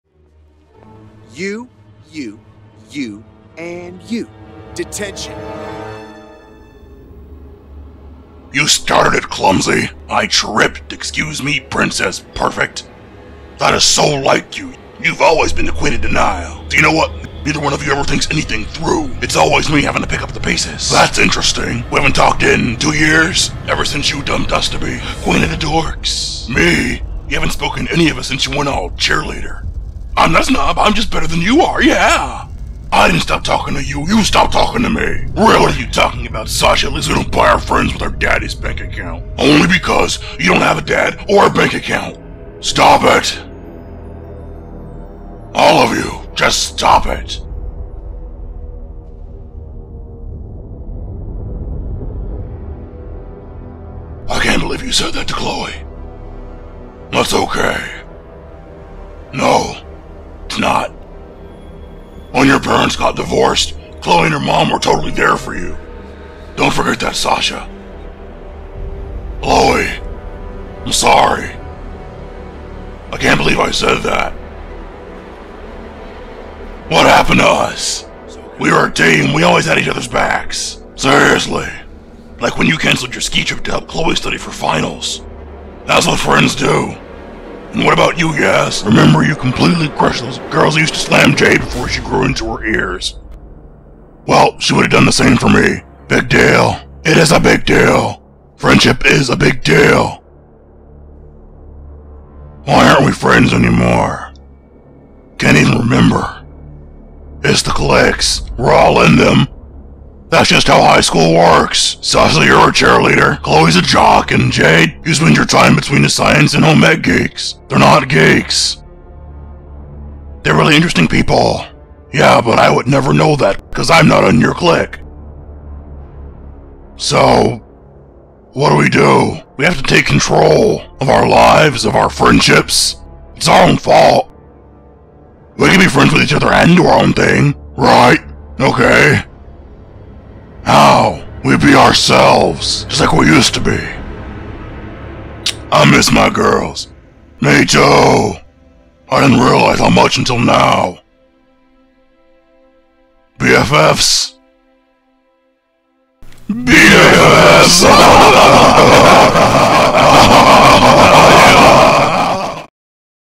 Bratman
Two years after starting high school, the girls are growing apart